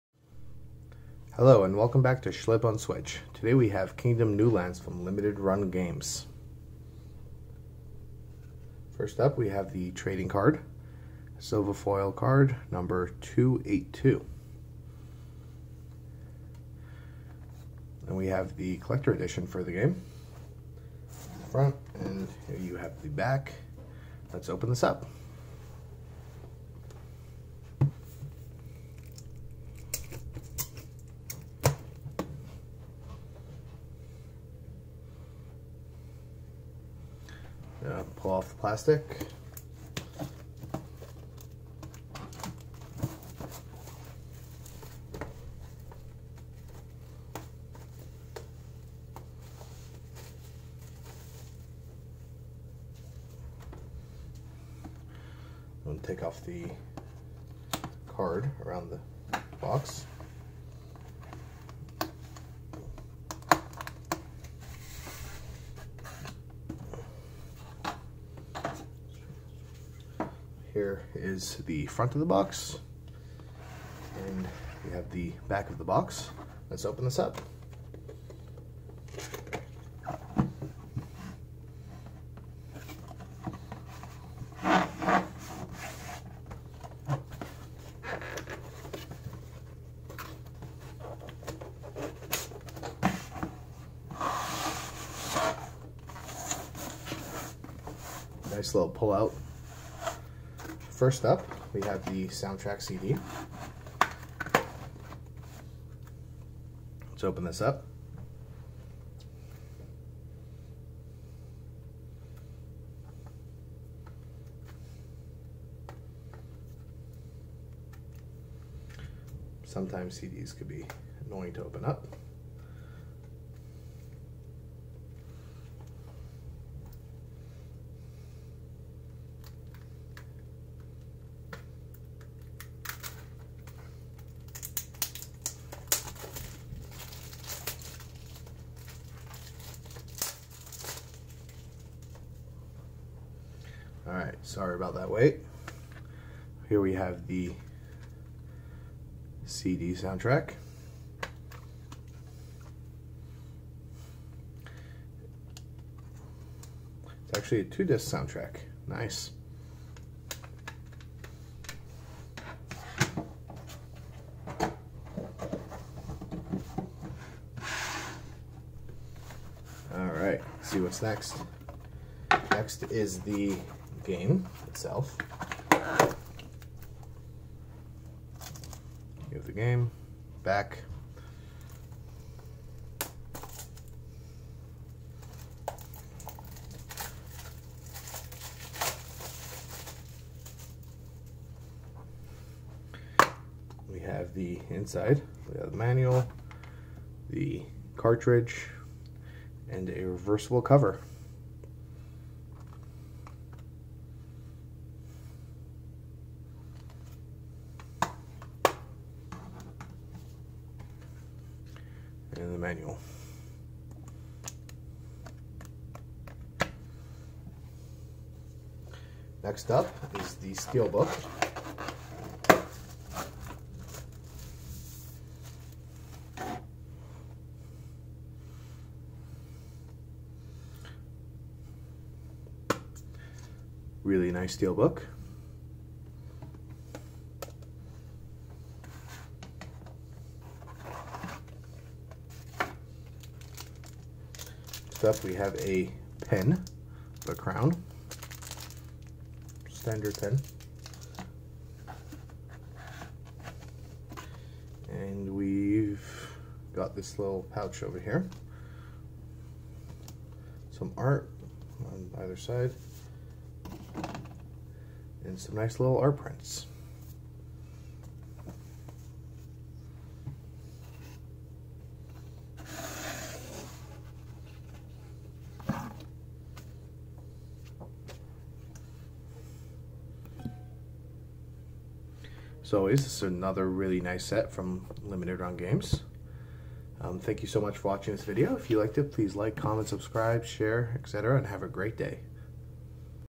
Kingdom New Lands Unboxing (Limited Run Games) Nintendo Switch
For more Switch fun be sure to check out my Twitter